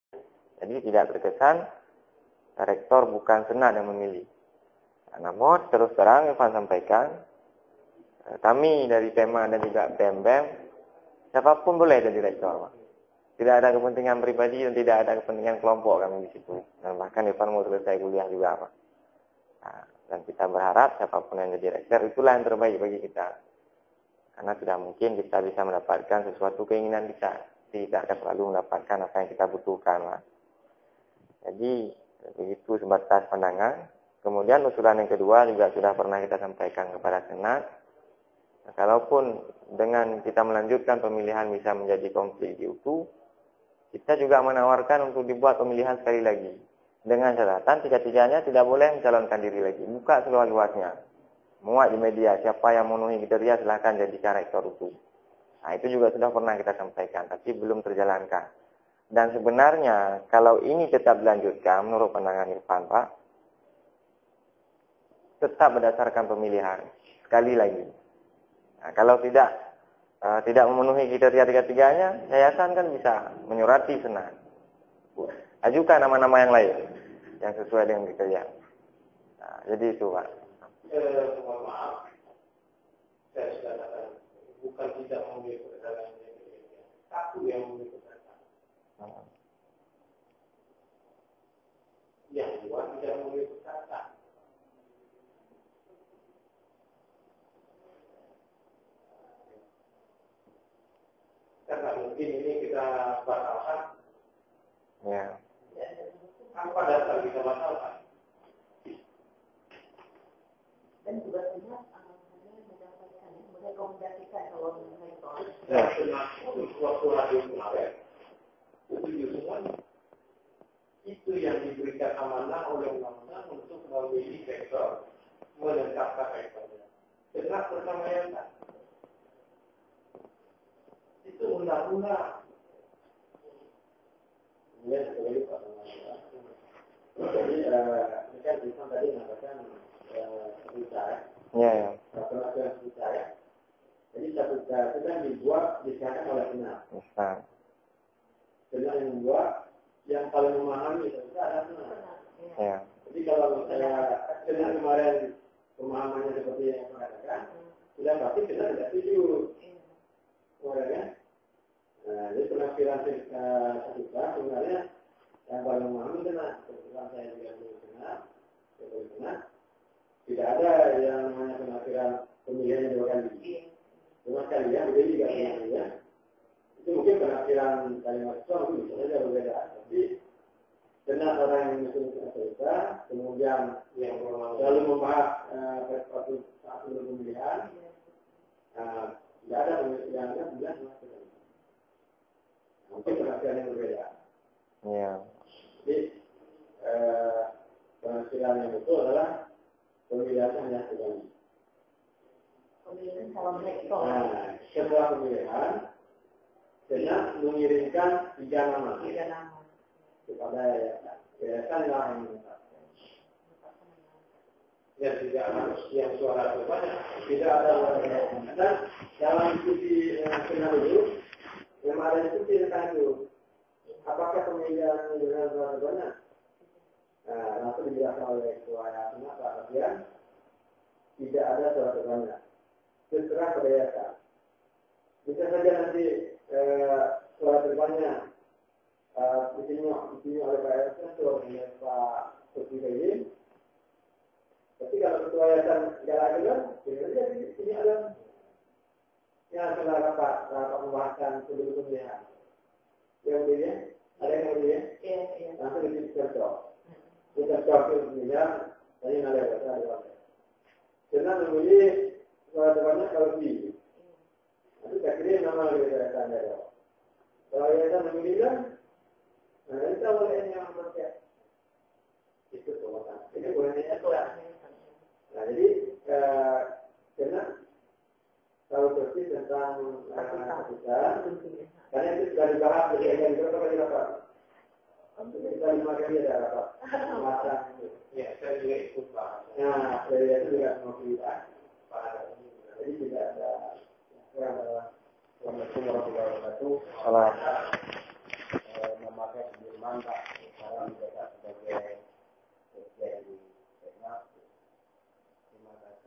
GSF liputan IS pertemuan mahasiswa UTU dgn Yayasan jum'at 26-8-11 part 6.wmv
Pertemuan Perwakilan Mahasiswa dengan Pengurus yayasan yapentu jopah diruang Asisten 1 Rusmahdi, SH sekali ketua yayasan yapentu Jopah yang baru,  pertemuan yang berlansung pada hari jum'ata pukul 09.00 wib sebelum pelantikan rektor pada pukul 15.00 wib siang tanggal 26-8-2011 pada bulan puasa. dalam dialog tersebut pihak yayasan tetap bersikukuh melantik Ir. Malik ali sebagai rektor, walaupun pada saat pemilihan rektor malik ali mendapat suara 0 (nol). inilah salah satu hukum rimba yang dipraktekkan yayasan.   apa hukum, statuta yang disodorkan mahasiswa tetapi tidak di gubris oleh yayasan.